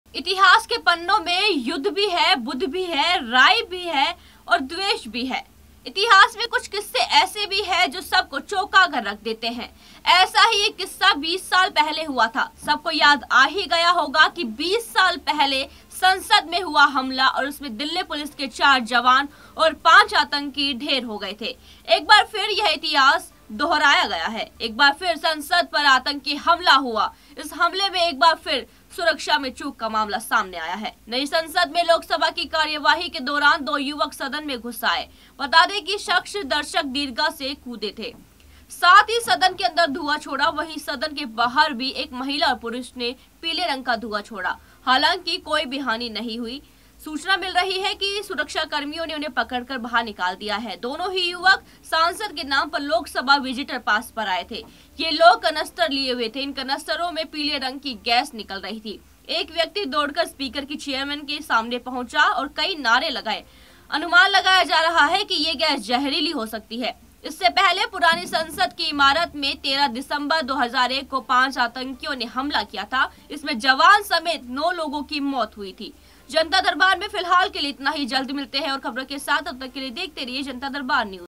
एक बार फिर संसद पर हुआ आतंकी हमला
20 साल पहले भी हुआ था हमला
4 जवान और 5 आतंकी ढे़र हुए थे ढे़र
सदन में छोड़ी जहरीली गैस
दो युवकों ने किया सदन पर हमला